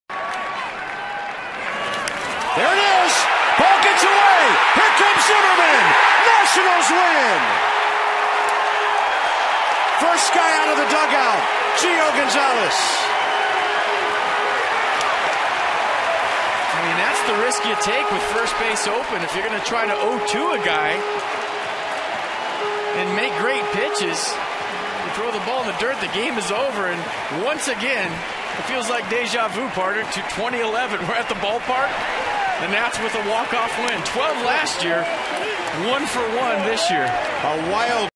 Nats' walk-off wild pitch 04/12/2012 (HD)
After Alfredo Simon bounces a pitch in the dirt, Ryan Zimmerman hustles home to score the game-winning run in the tenth